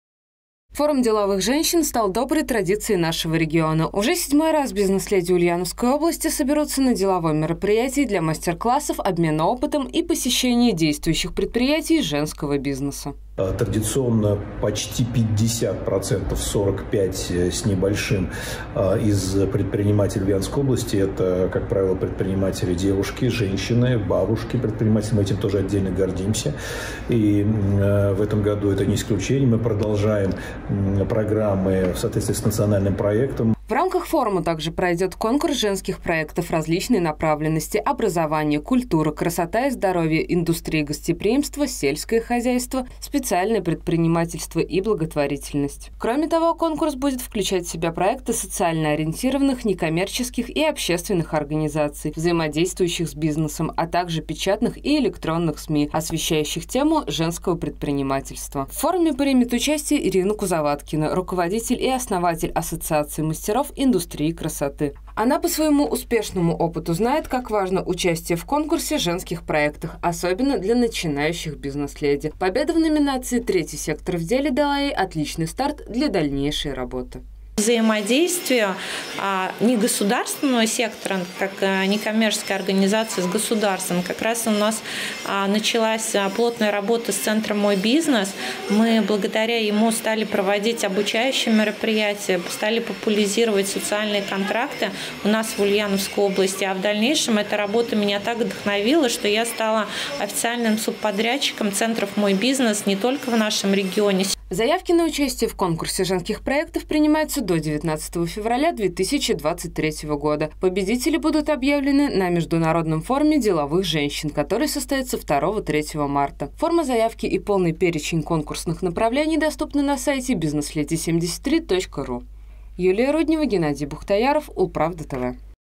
В Ульяновской области выберут лучшую бизнес-леди
В Ульяновске стартовал приём заявок на конкурс женских проектов в рамках Форума деловых женщин. Принять участие могут предпринимательницы и те, кто пока только собирается ею стать. Итоги конкурса подведут 2-3 марта.